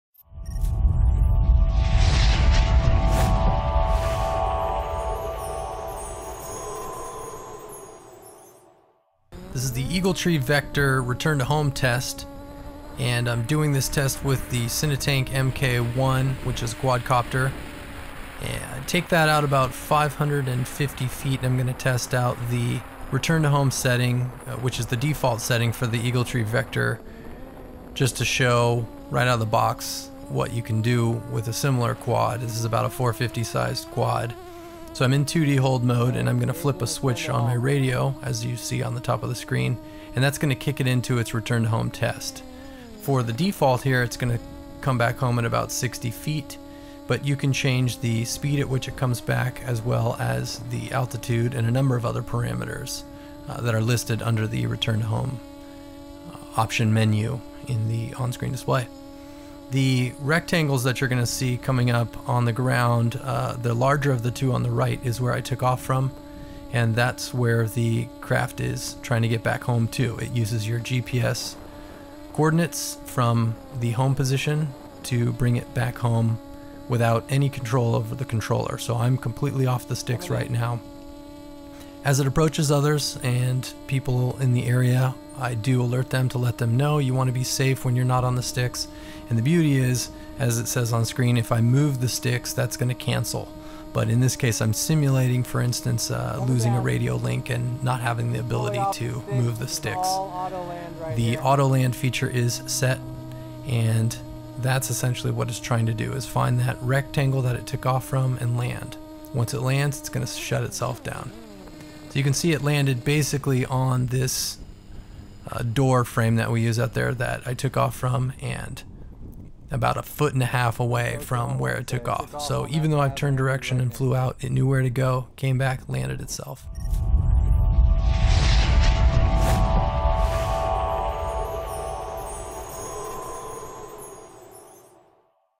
GSTV - Eagle Tree Vector - Return To Home (RTH)
Available at ReadyMadeRC

Showing the Vector with my Cinetank on a RTH test. This is an amazing On Screen Display (OSD) / Flight controller for multicopters.